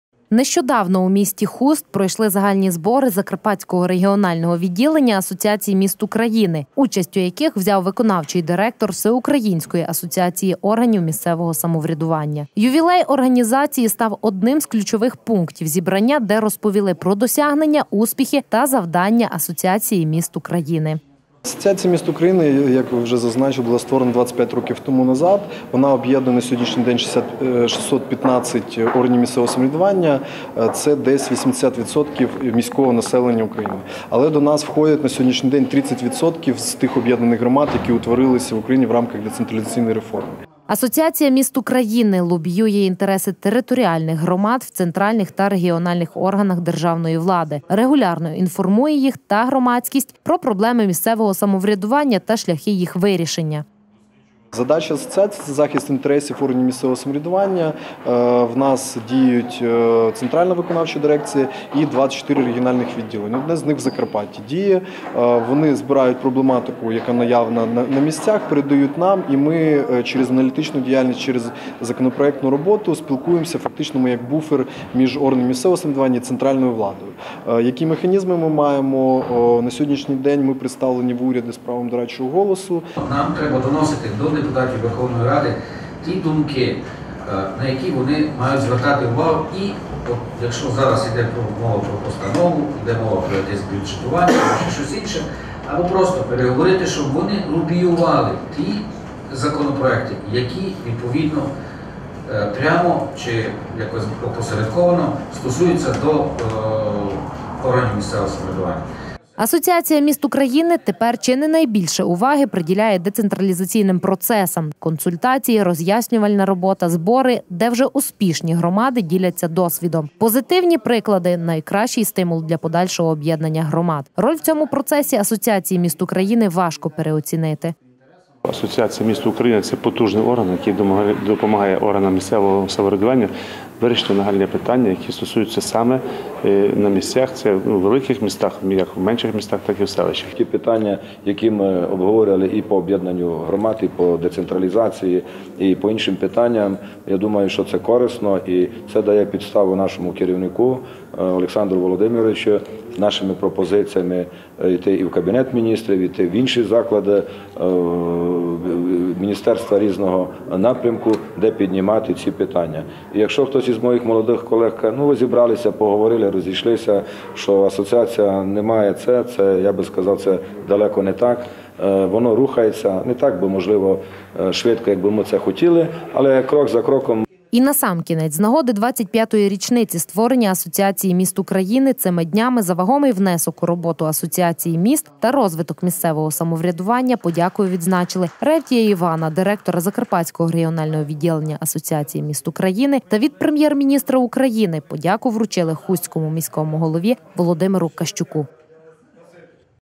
Асоціації міст України – 25!
Асоціація міст України практично ровесниця незалежної України. Заснована в червні 1992 року, і на той час до її складу входило 35 міст. Ще тоді було визначено основну мету діяльності Асоціації - підтримка курсу ринкових перетворень, розширення зв'язків між містами на ґрунті економічного співробітництва, захист законних прав та інтересів членів Асоціації в органах влади та управління, взаємодія з владними структурами і громадськими об'єднаннями в справі перетворення України на країну з соціально орієнтованою ринковою економікою.